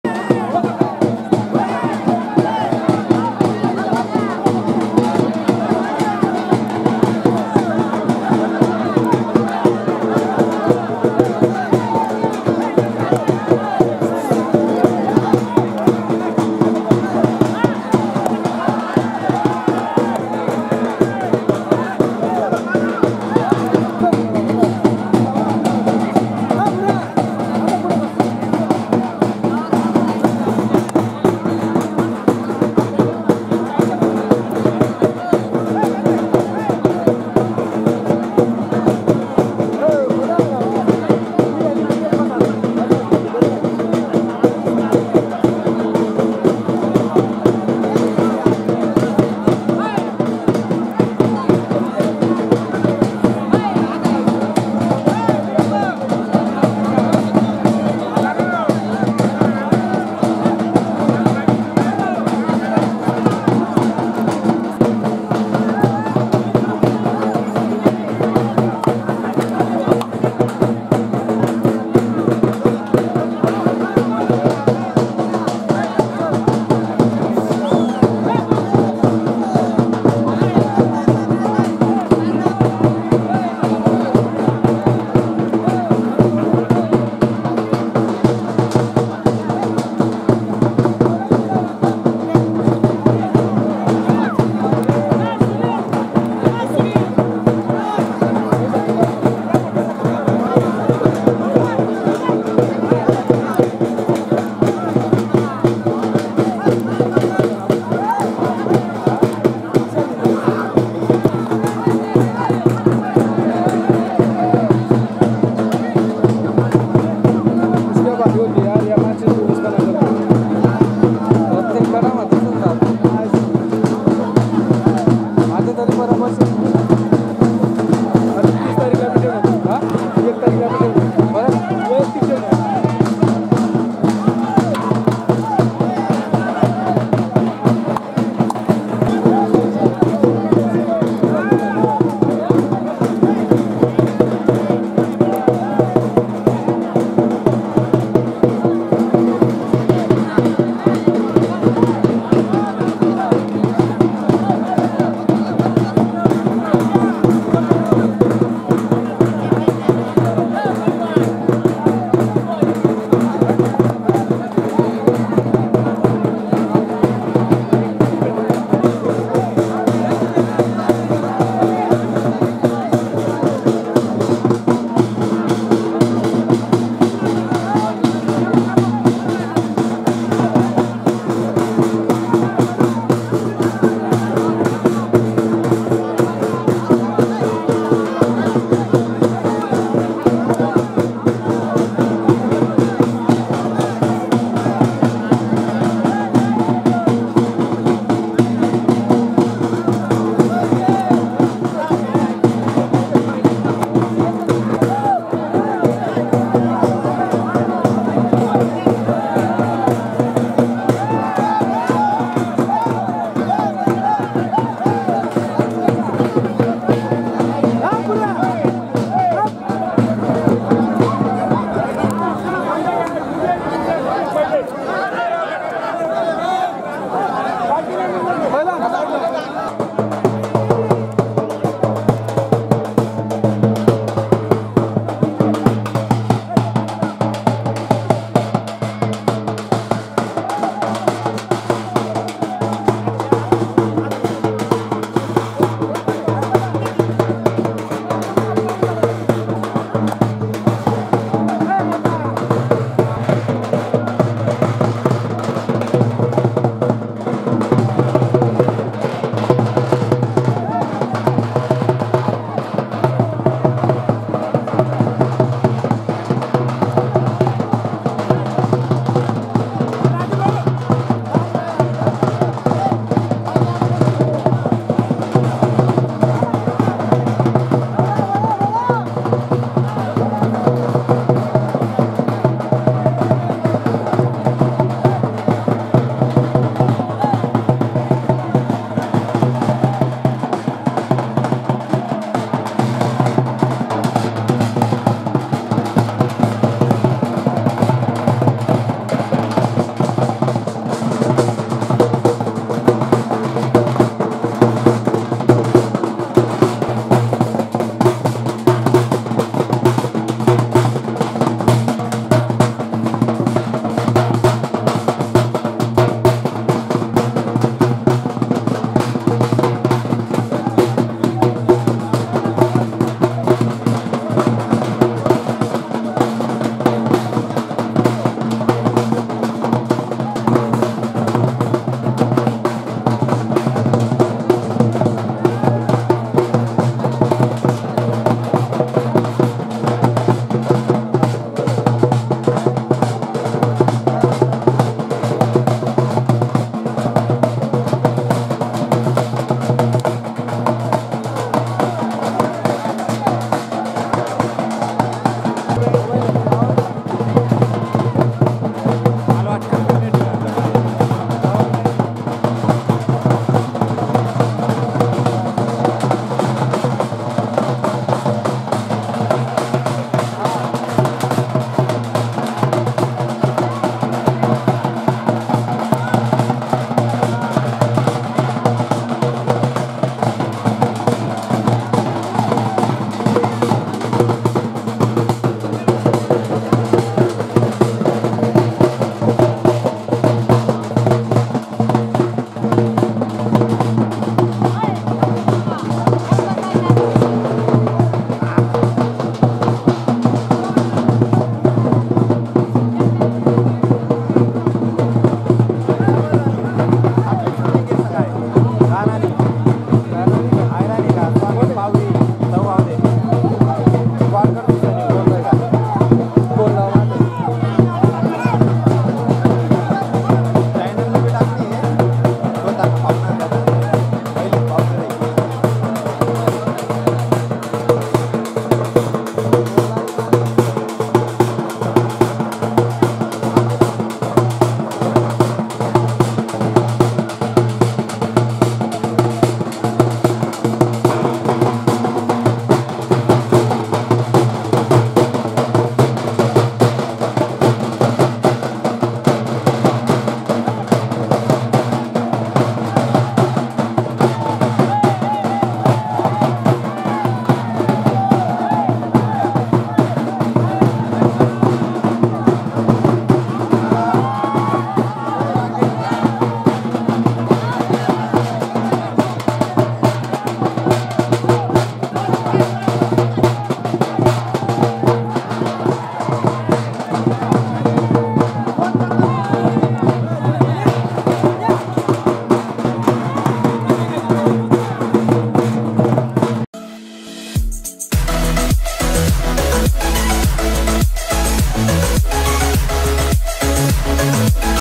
सोनू पहलवान (शेवाळे) V/s योगेश पहलवान (निळगव्हाण) उडाणे कुस्ती दंगल | Sonu V/s Yogesh At Udane
संदर्भ : कुस्ती दंगल Youtube चॅनल
जर आपल्याला व्हिडिओस आवडले असतील तर आपल्या चॅनल ला sabscribe करा आणि विडिओ ला लाईक करा धन्यवाद
आमचा whats app नंबर : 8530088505

Follow  Our other social platform ID